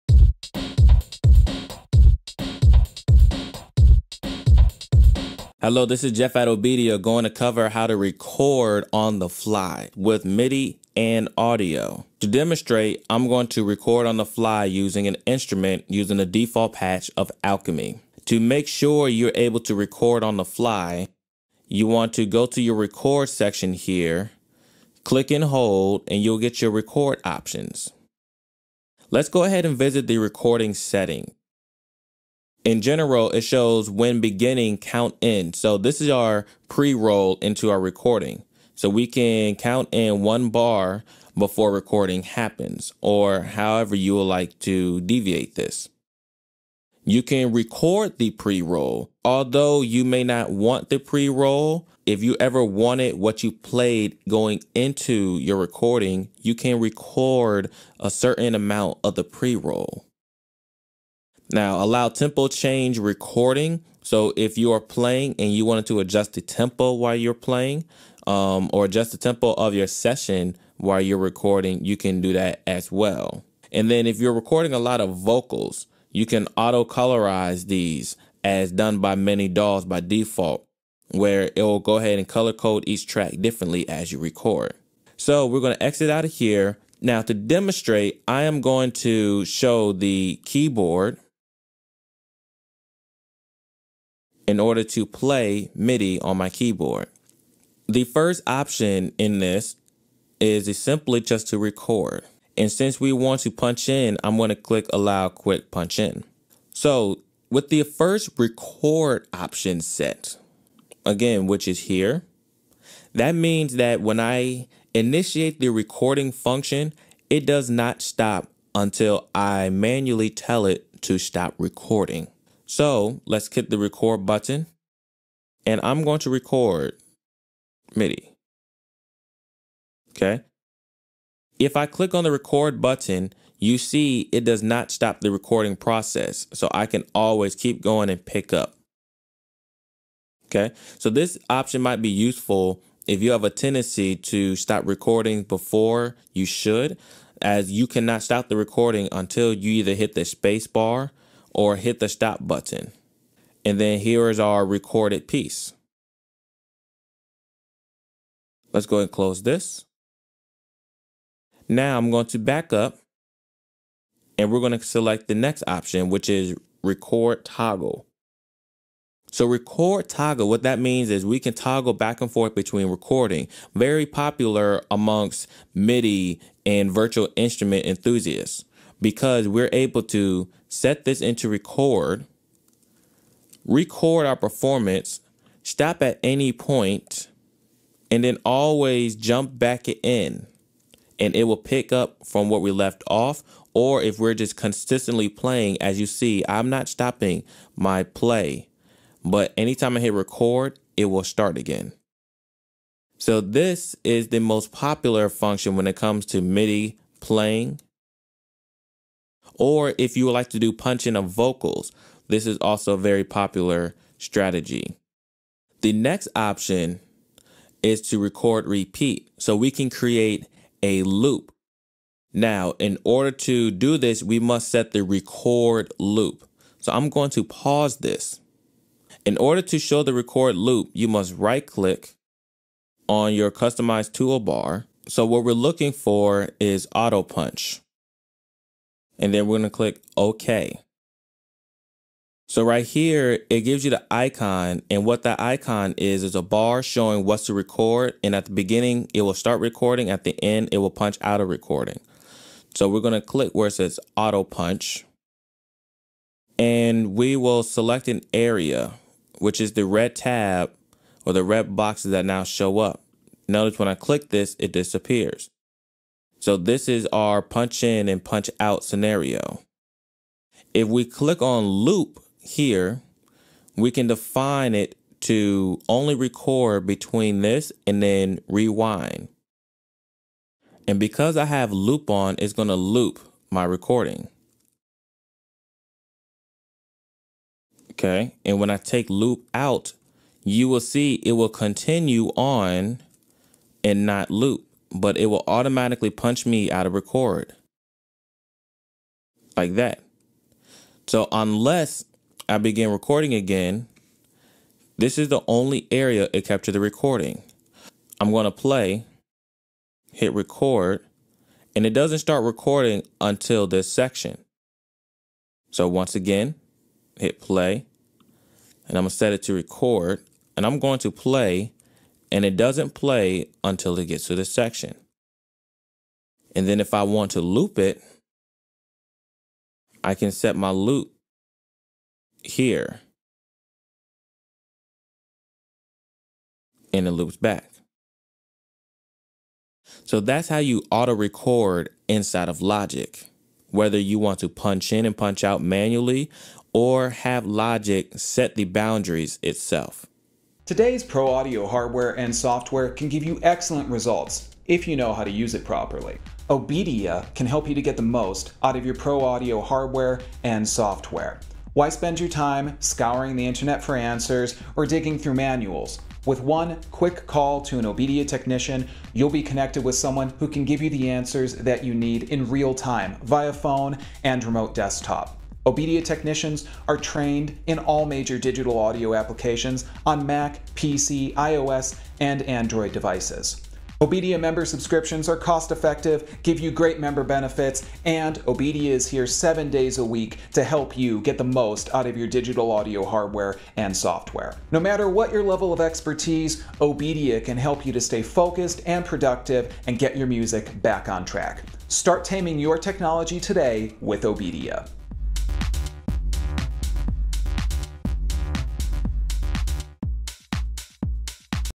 Record on the fly in Logic Pro
In this video, we will show you how to record on the fly in Apple Logic Pro.